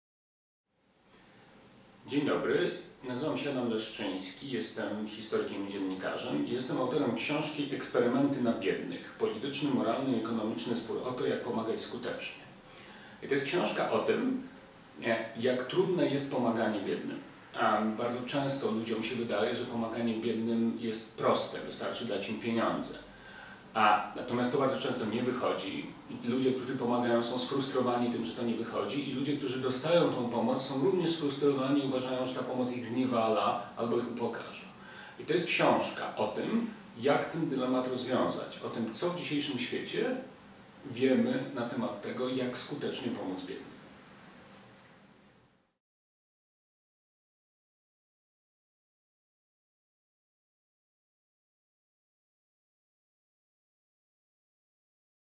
Wydawnictwo KP: ADAM LESZCZYŃSKI | "Eksperymenty na biednych"
Dzięki “eksperymentom na biednych” - wedle metody zaczerpniętej chociażby z badań farmakologicznych - świat ponownie uwierzył w pomoc rozwojową, w to że dzięki skutecznym i efektywnym programom uda się w końcu zlikwidować skrajne ubóstwo. Z drugiej strony, nowe metody oznaczają też nowe kłopoty, bo czy na pewno eksperymenty dają ostateczną odpowiedź na pytanie, jak i komu pomagać?

Patronat:
Tok.fm
Le Monde Diplomatique Edycja Polska